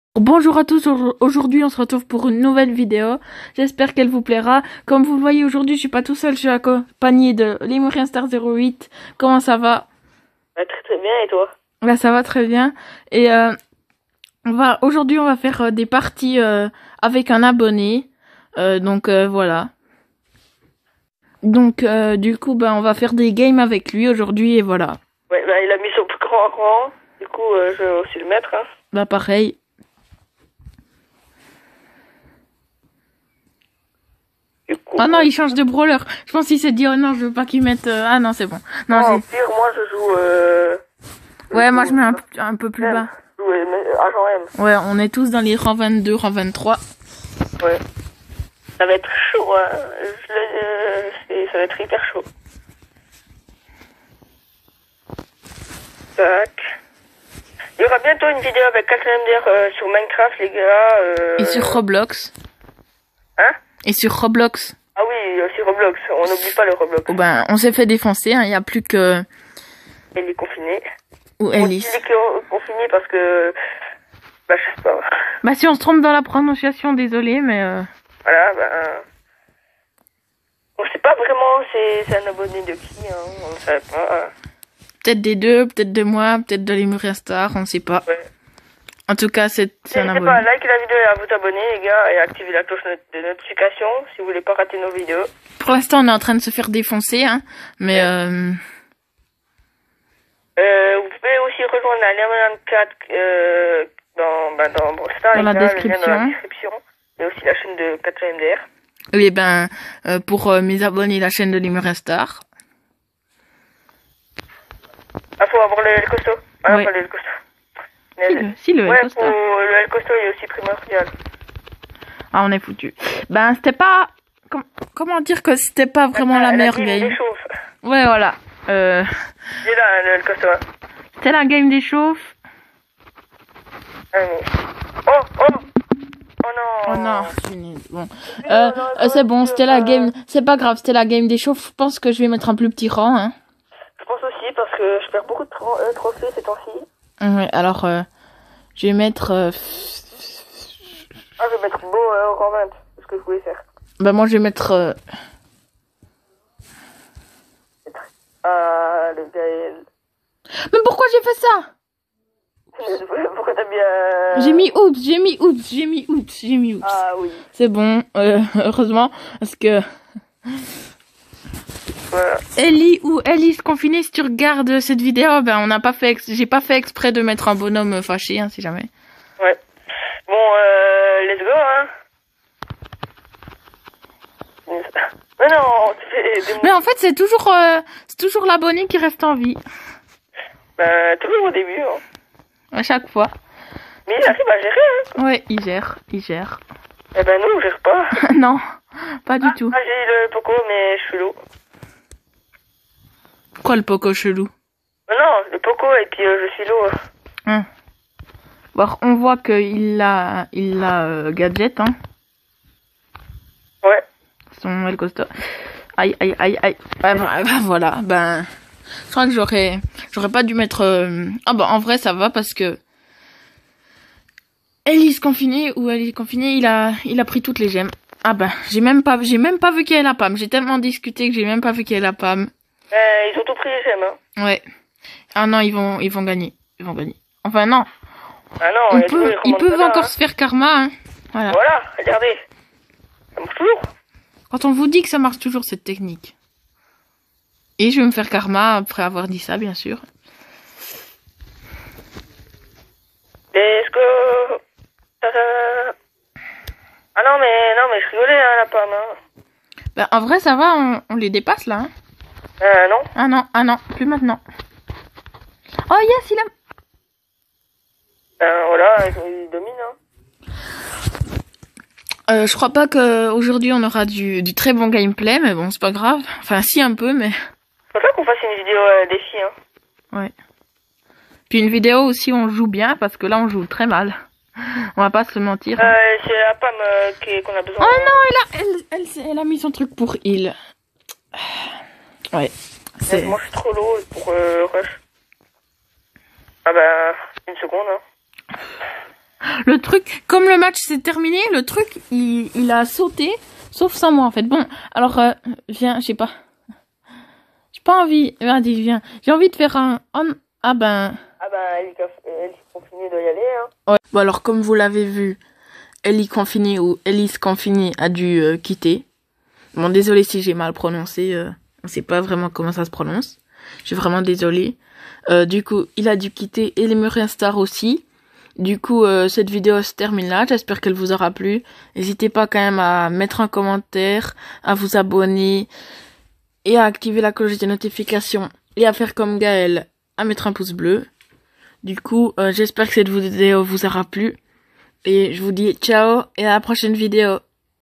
ON JOUE AVEC UN ABONNÉ AVEC LEMURIEN_STAR08
Aujourd'hui, on fait une petite vidéo, on va faire des games avec abonné avec Lemurien_star08